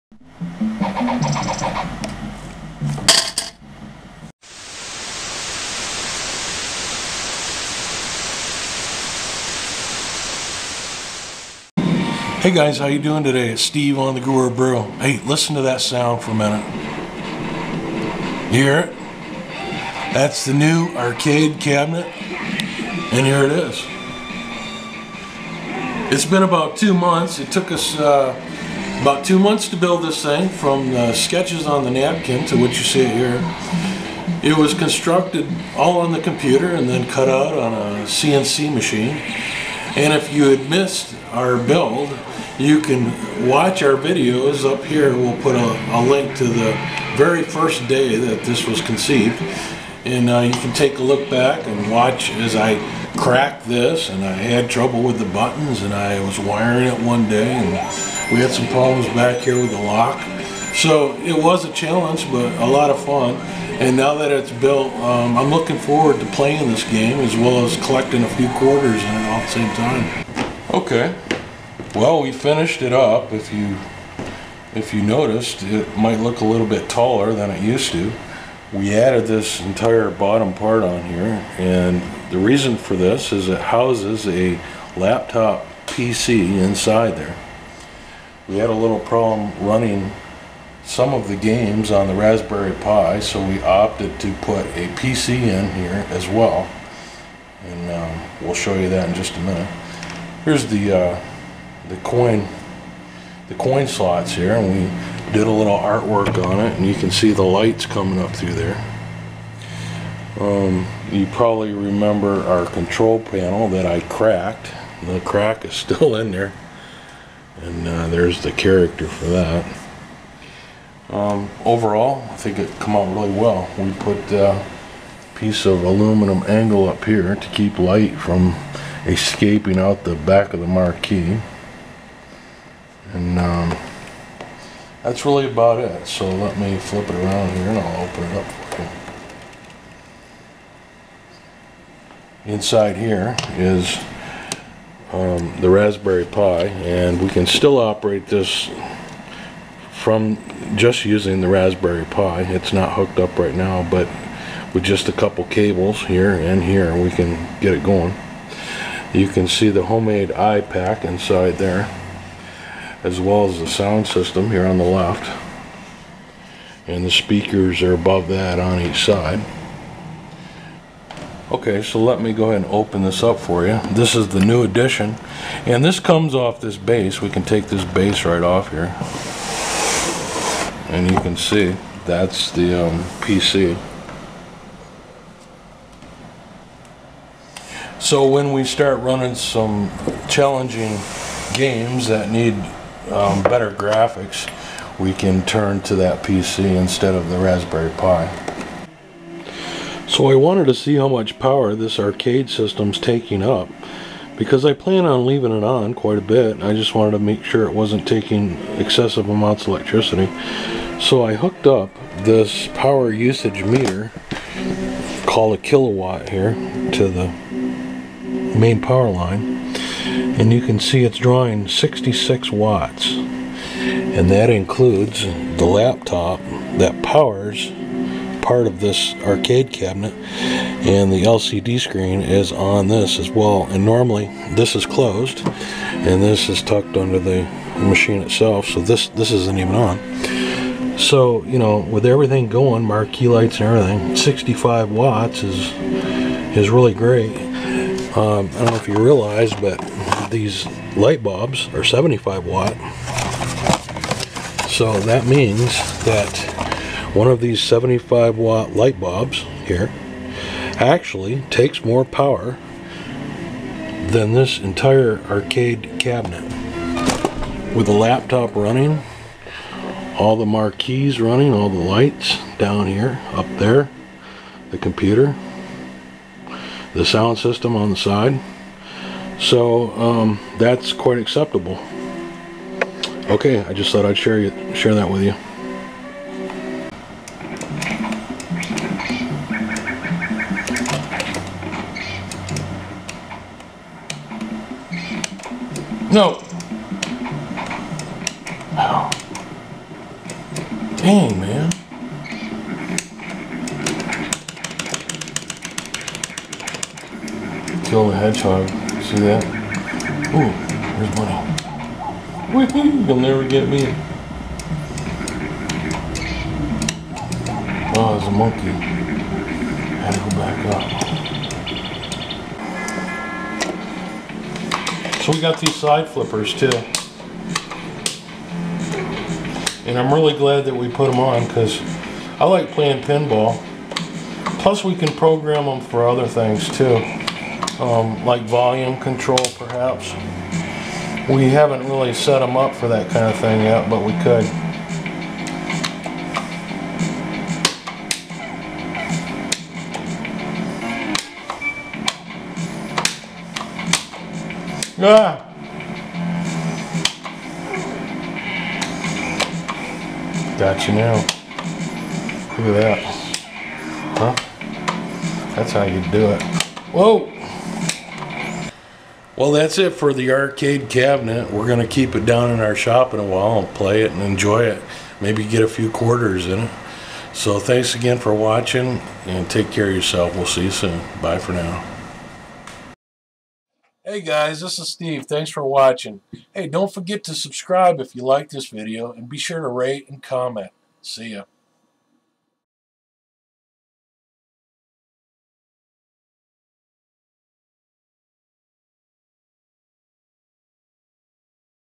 Video Game Arcade Machine Build - Final Reveal
We finally finished our DIY bar top arcade gaming cabinet and present you with the finished product in this video. This machine was constructed using CAD/CAM from the ground up. Check our channel for more videos documenting every aspect of this build.